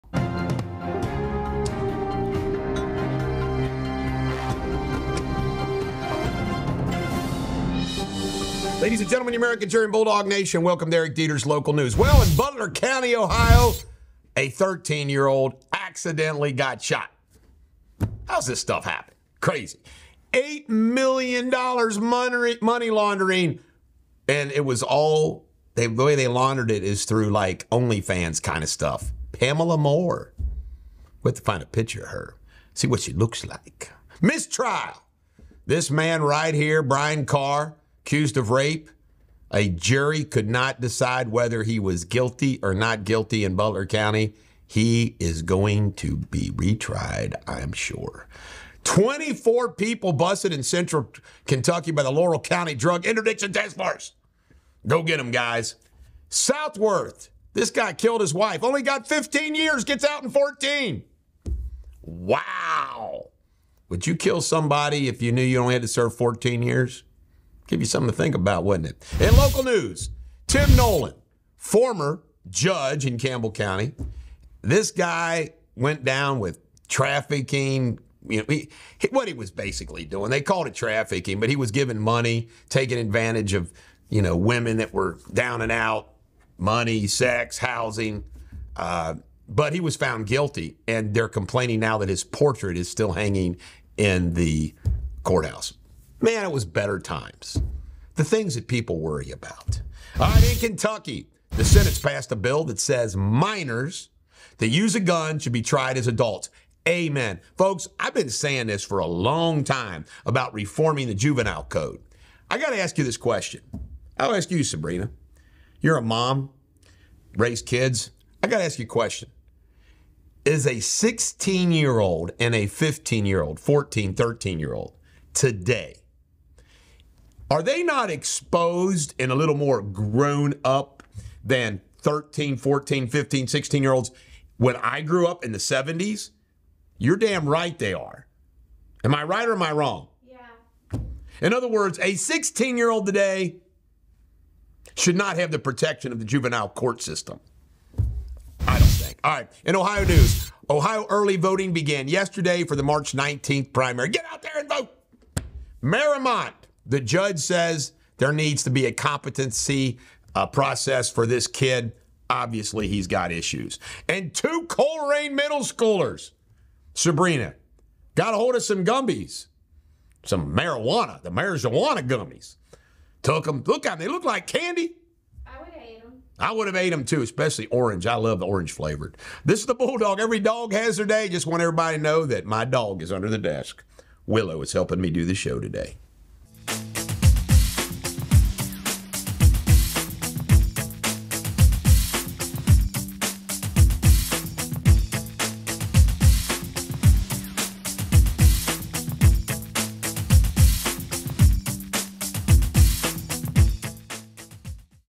Cannabis Gummies Trigger Health Scare at Colerain Middle School | Eric Deters Local News
Cannabis Gummies Trigger Health Scare at Colerain Middle School | Eric Deters Local News | February 22, 2024

Stay informed with the latest developments in your area on today's episode of Local News. From shocking incidents to legal proceedings, we've got you covered. Tune in as we discuss the accidental shooting of a teen in Butler County, an $8 million laundering scheme, and the outcome of a mistrial. Plus, learn about a major drug bust resulting in 24 arrests and the controversial release of Southworth after just 14 years for killing his wife. Explore local issues such as Tim Nolan's picture remaining in the courthouse post-sentencing and the debate over minors being tried as adults for gun-related crimes in Kentucky. In Ohio, get updates on the commencement of voting and evaluations of school threats in Mariemont and Colerain middle school's cannabis gummy alert.